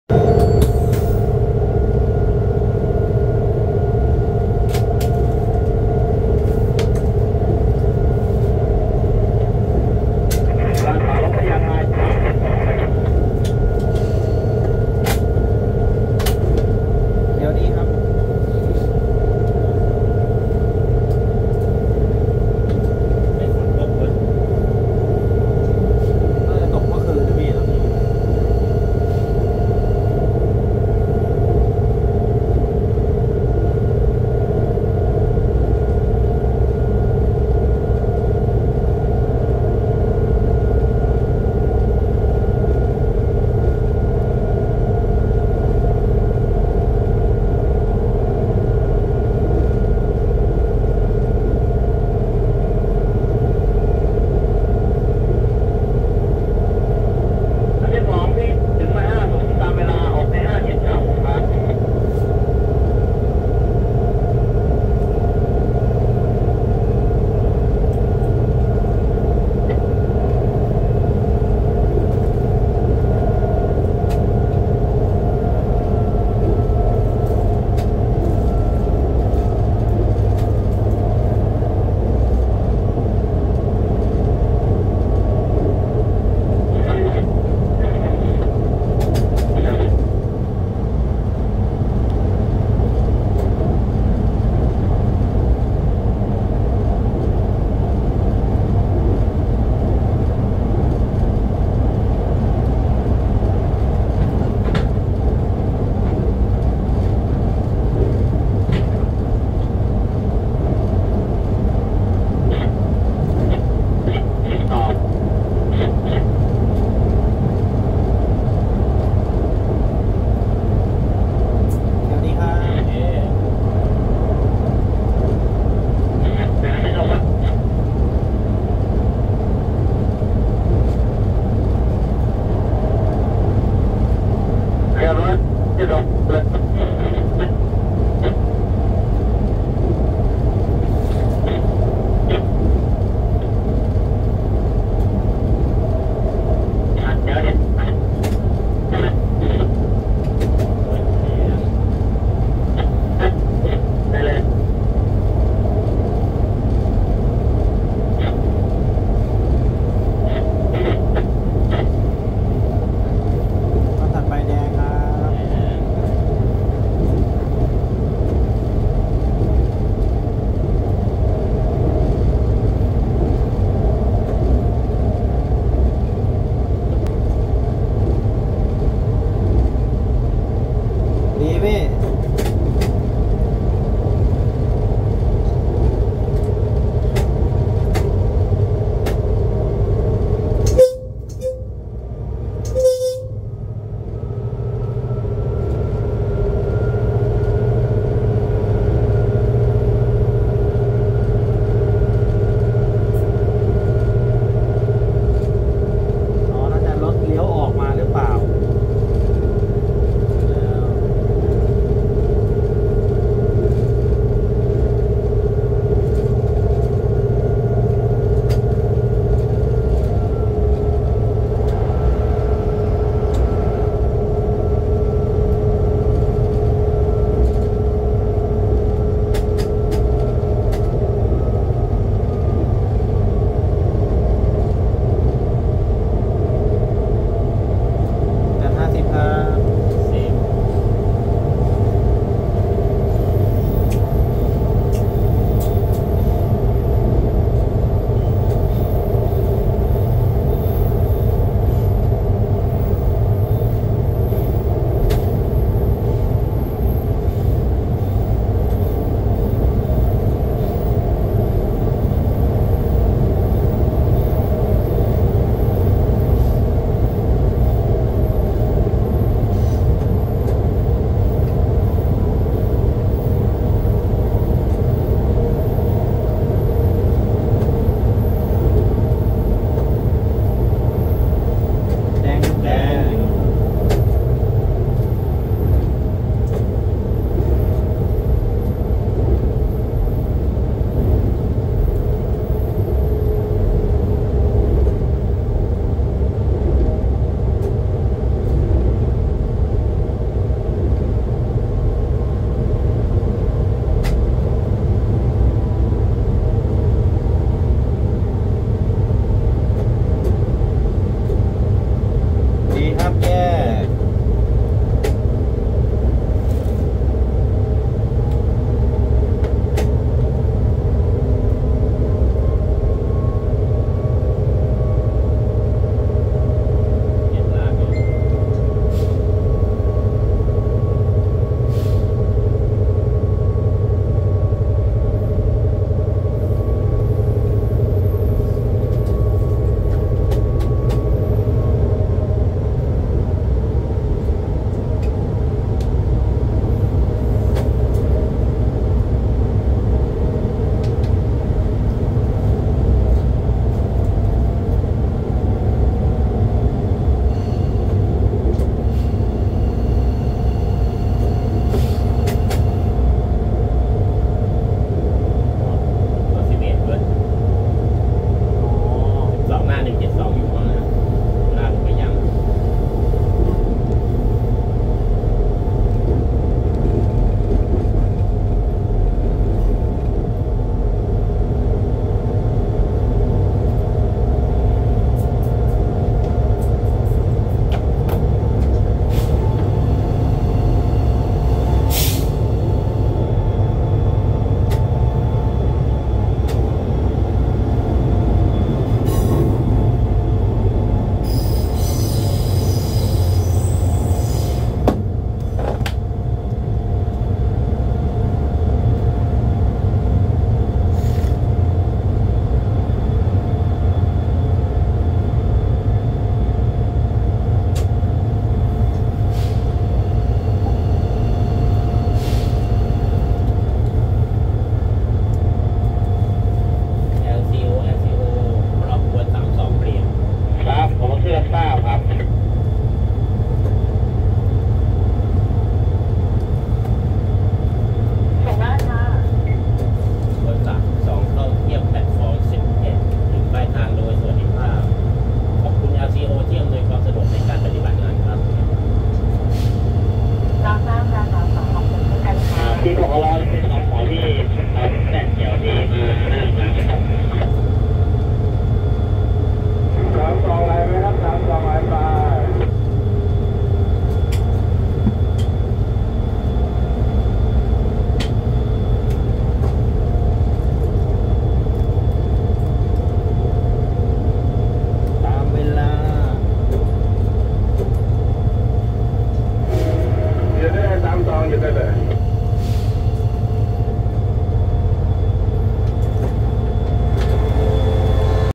ขบวนรถไฟสายใต้ | วิ่งทางรถไฟยกระดับเข้าสถานีกลางกรุงเทพอภิวัฒน์
ขบวน 32 ออกบางบำหรุ
วิ่งขึ้นทางยกระดับเข้าสถานีกลางกรุงเทพอภิวัฒน์